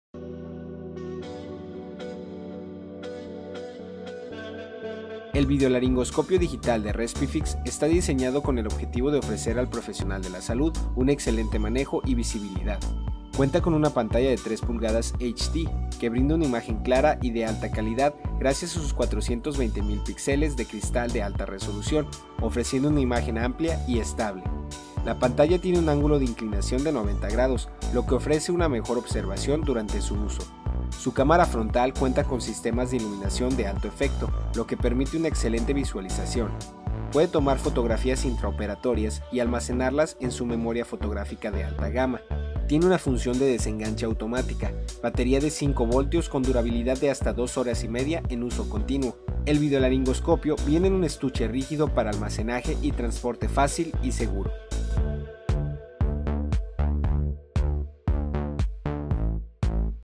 Videolaringoscopio
Conoce mas acerca de como este producto funciona y ayuda al personal medico en sus actividades del día a día, no olvides contactar a tu distribuidor respifix autorizado.